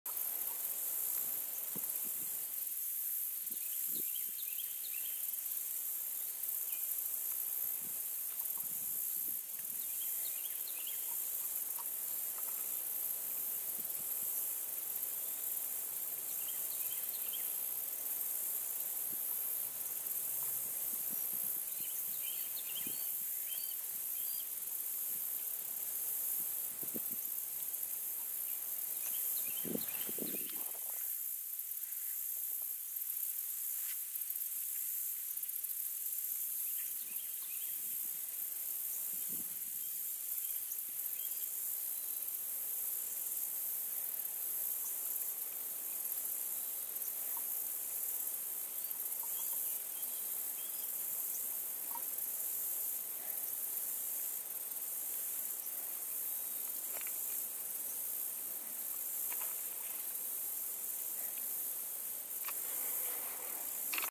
Wildlife
Belted kingfisher was very difficult to film because of its eratic and shy nature.